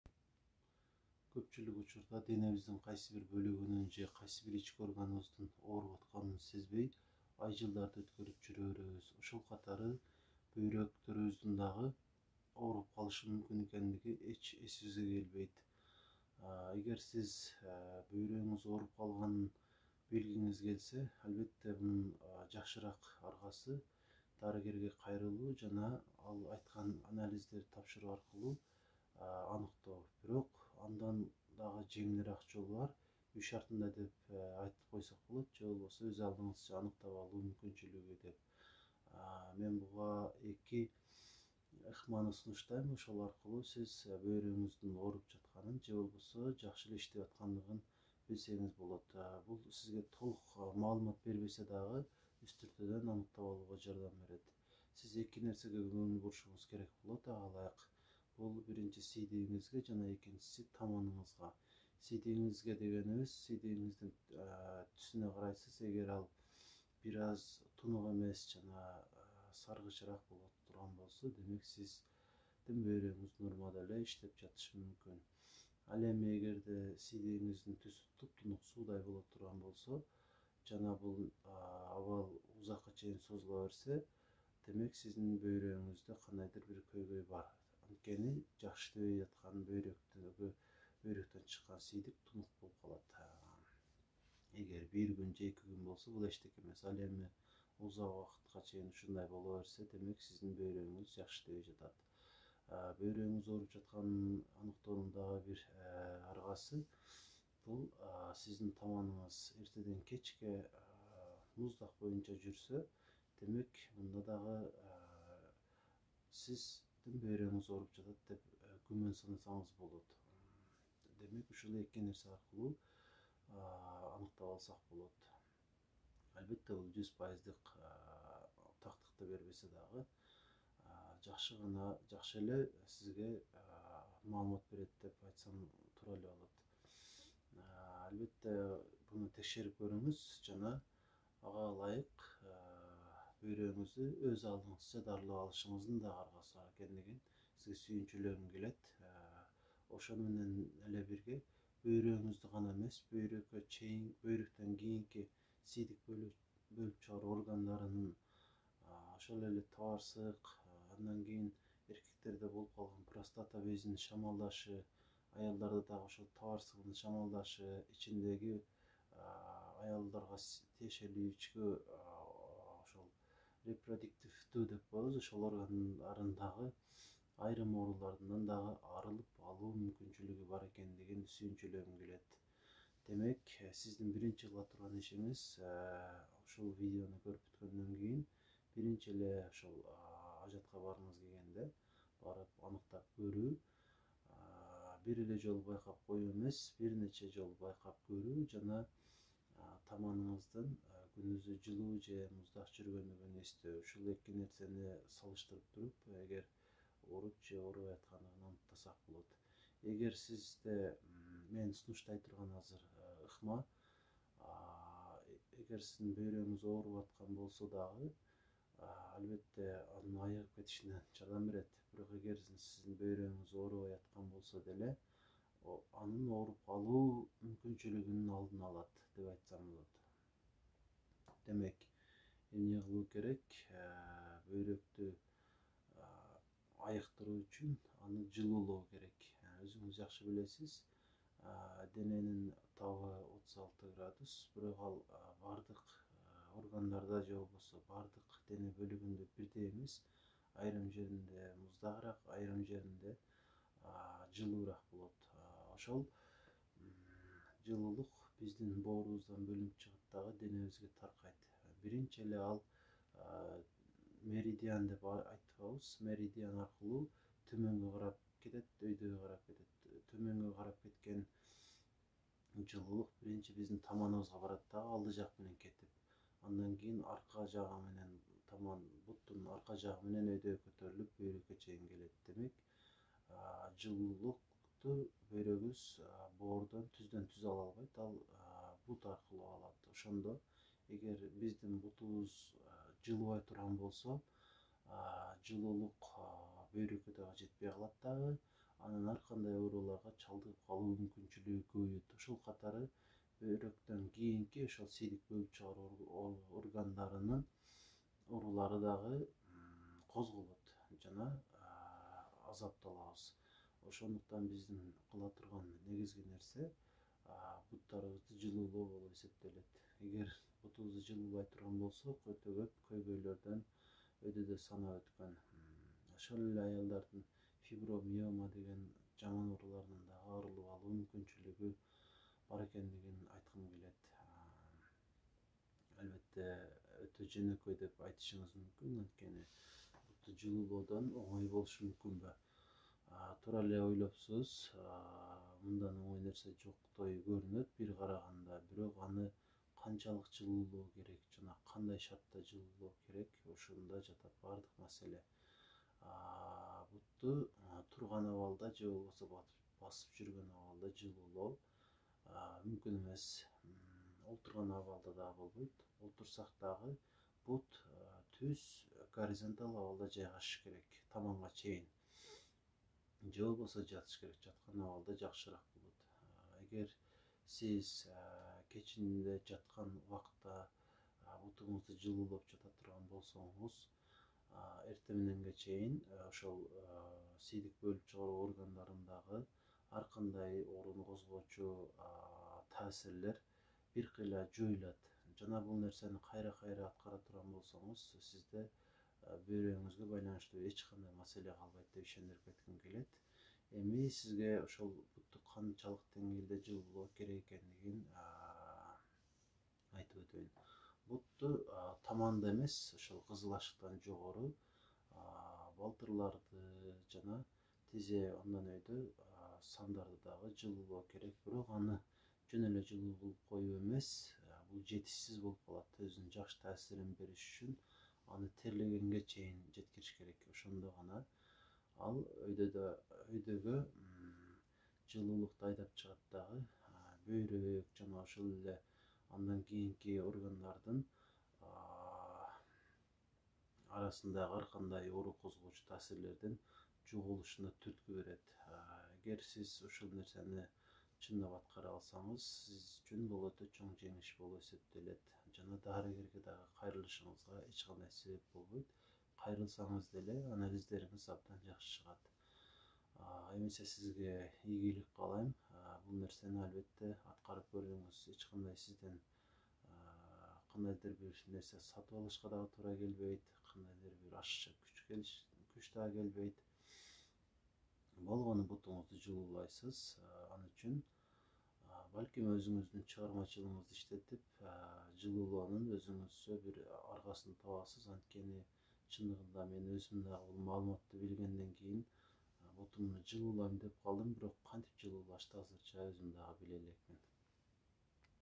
БӨЙРӨК ООРУСУН ДАРЫЛОО / БАРДЫК БАЛЭЭ МУЗДАК ТАМАНДАРДА
Бөйрөктүн жакшы иштөөсүн шарттоочу 2 нерсе бар. Алар жетиштүү өлчөмдө суу ичүү жана таманды жылуулоо.

Видео жакса каналга катталып коюшуңузду суранам. Анткени, алдыда сизди дагы толтура пайдалуу маалыматтар күтүп жатат.